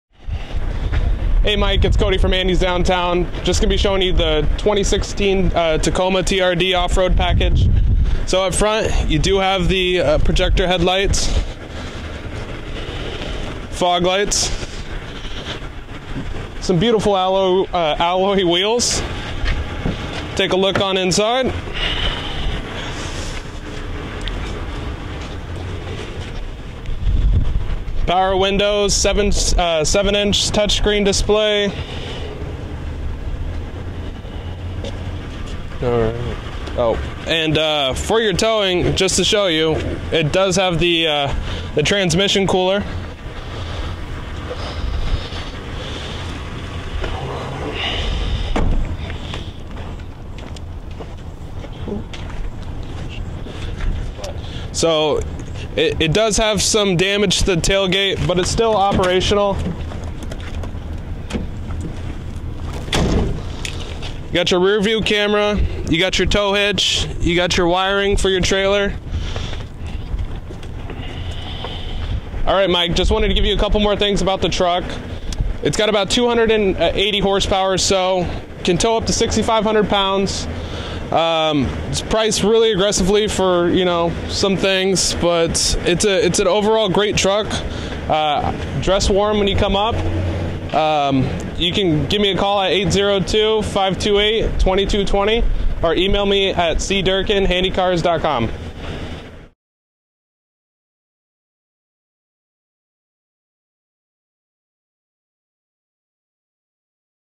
2016 Toyota Tacoma for Mike from Cody at Handys Downtown
This video is about 2016 Toyota Tacoma for Mike from Cody at Handys Downtown